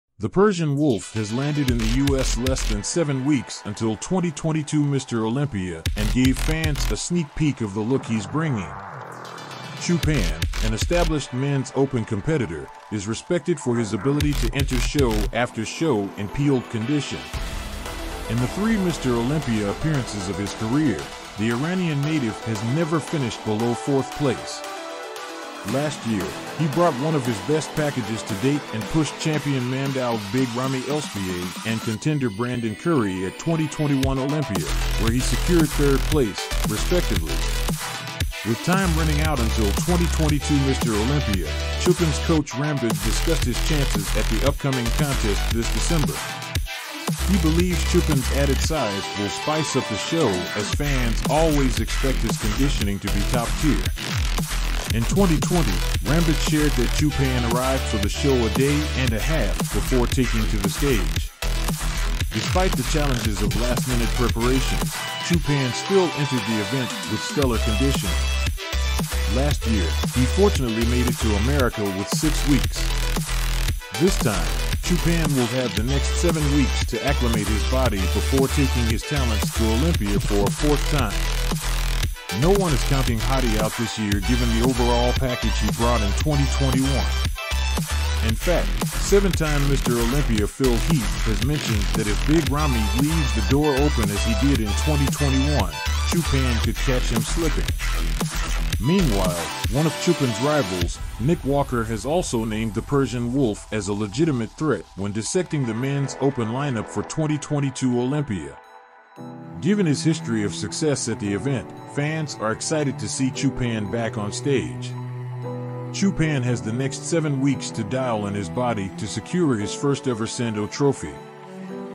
The World's Greatest Body Hadi Choopan Road to Mr. Olympia 2022
This is the Road to Mr. Olympia 2022 where Hadi Choopan trains and prepares for the Mr. Olympia competition. Hadi is one of the world's greatest bodybuilders and this video will show you his intense training regime and what it takes to be the best in the world.